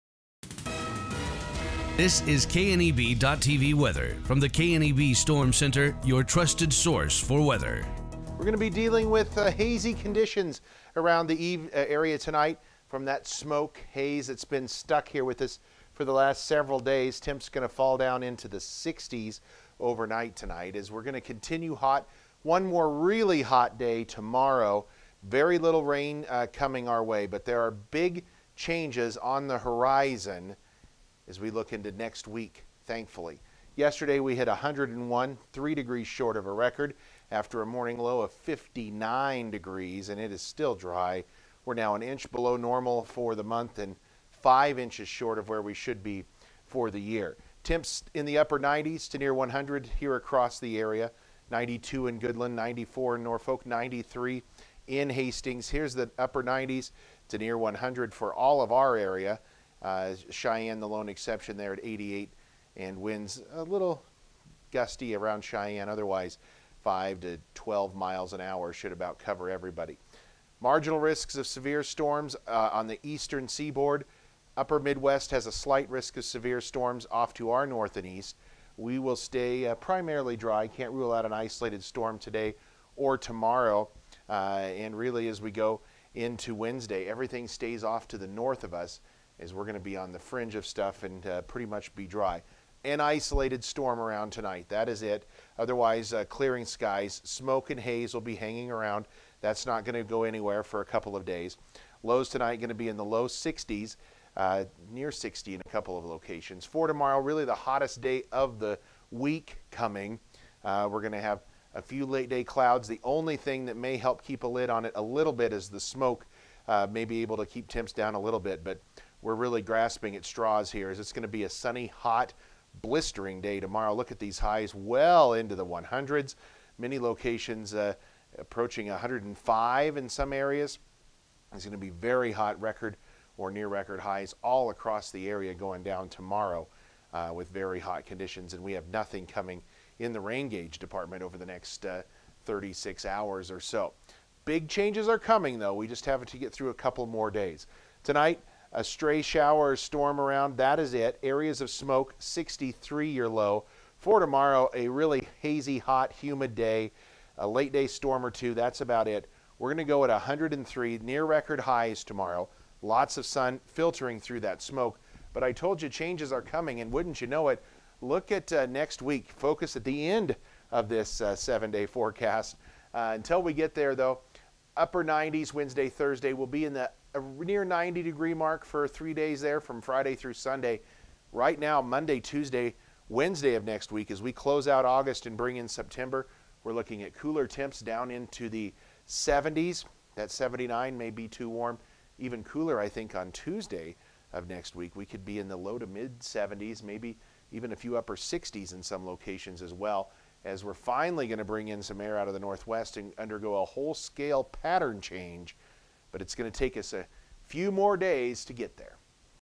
KNEB.tv Weather: August 24, 2020
Bill Boyer in with your full week forecast here on KNEB.tv Weather: